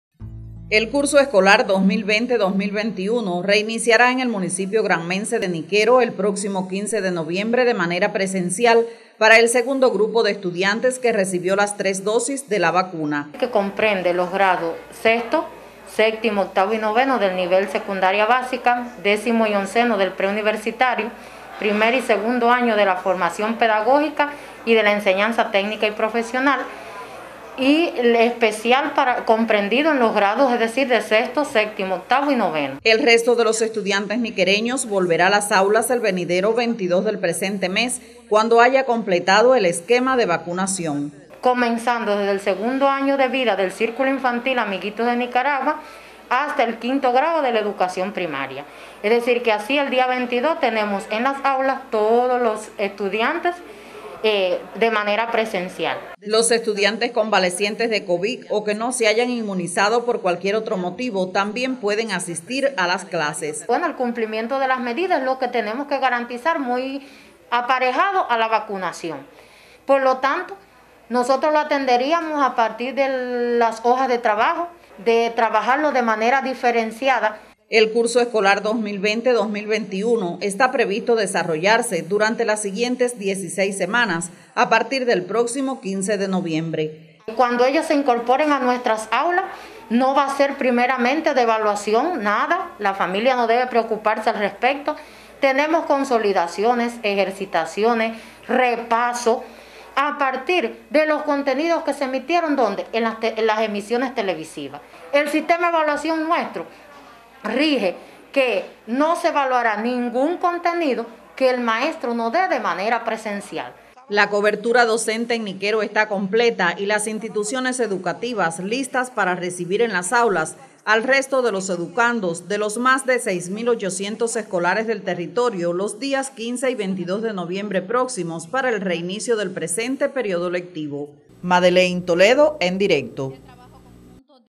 Preparan en Niquero reinicio del curso escolar 2020-2021 de manera presencial
En el costero municipio de Niquero se alistan las condiciones para que el segundo grupo de estudiantes reinicie de manera presencial las actividades del curso escolar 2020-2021.